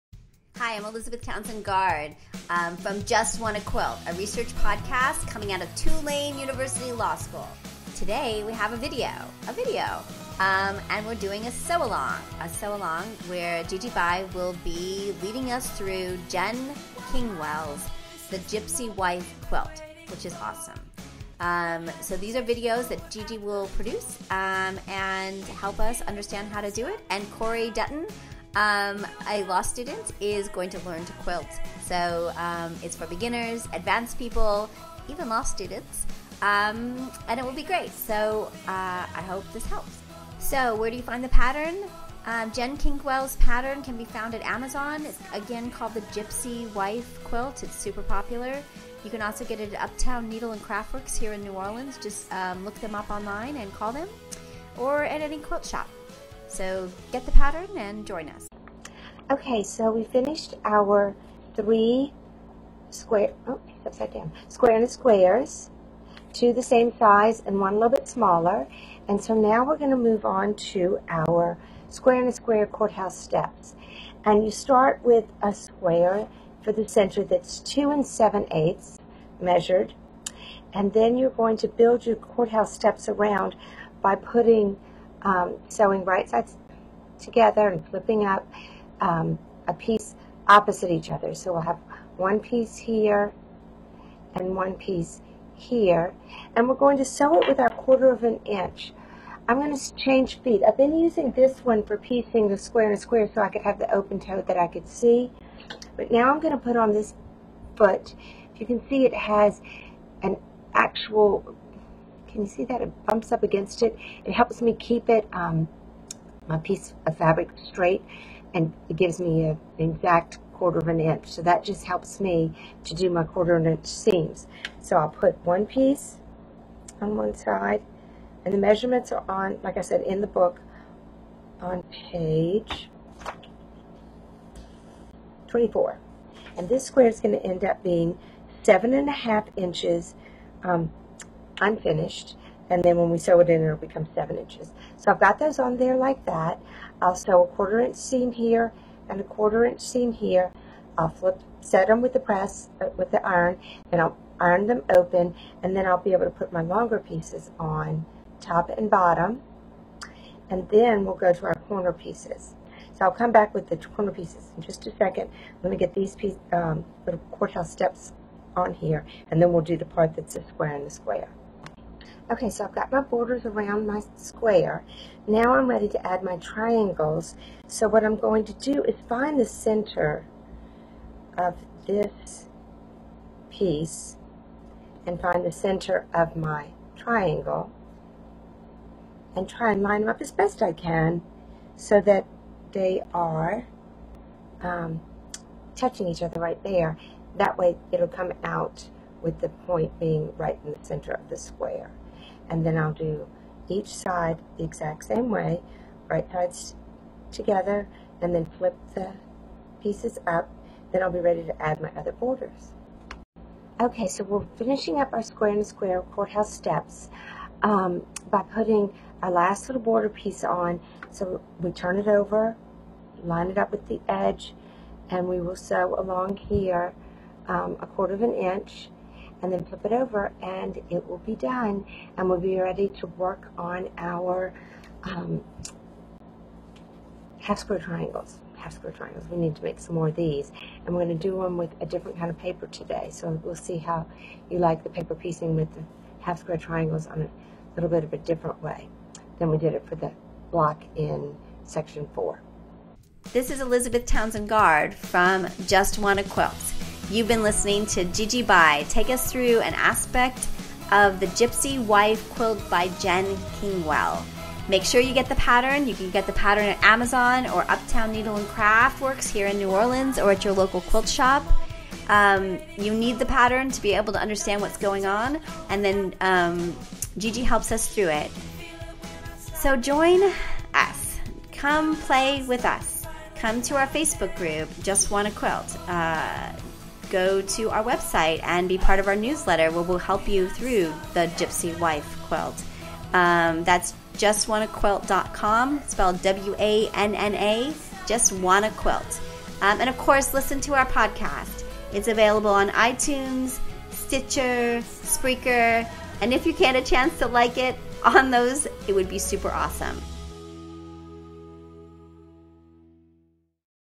Just Wanna Quilt- Gypsy Wife Square in a Square with Courthouse Steps
Gigi helps us create Section 5's Square in a Square with Courthouse Steps. Happy Quilting!